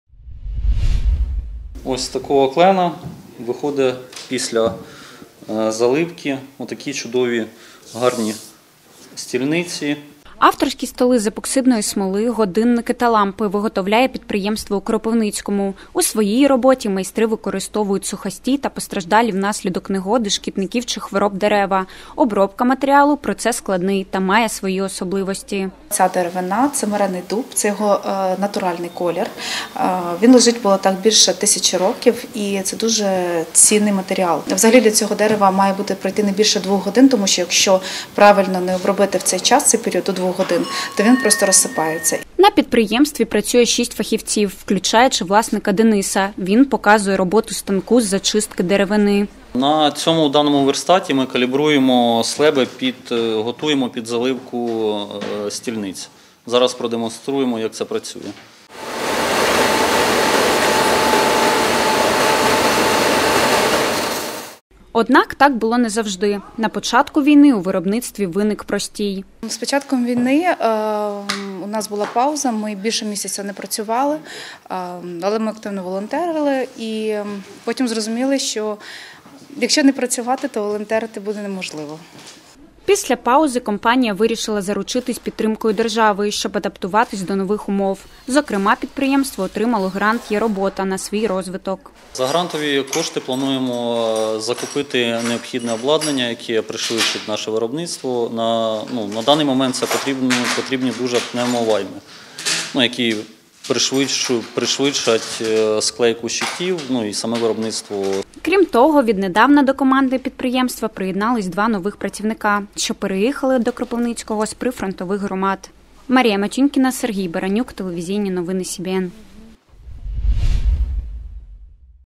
Кропивницьке підприємство створює вироби з тисячолітніх дерев | телеканал Вітер
У кропивницькій майстерні “Starwood” виготовляють ексклюзивні авторські роботи з дерева. Проте на початку війни підприємство перебувало у простої. Як вдалось відновити роботу та які особливості роботи бізнесу під час війни - дивіться у сюжеті.